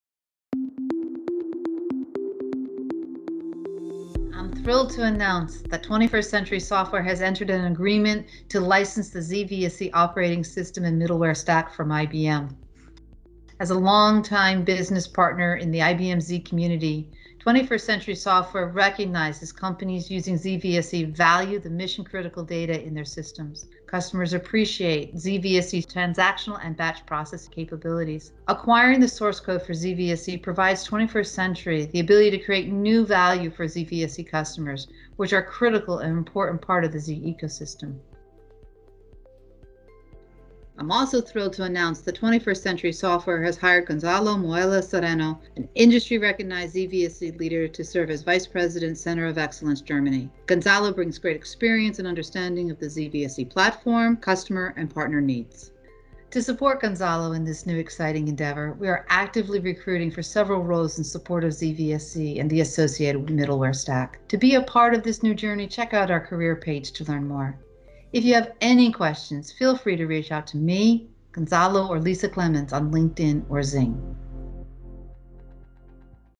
21st Century Software z/VSE
21st Century Software has entered into an agreement with IBM to license the most recent source code for the z/VSE operating system and middleware stack for IBM Z. Learn about the future of z/VSE here.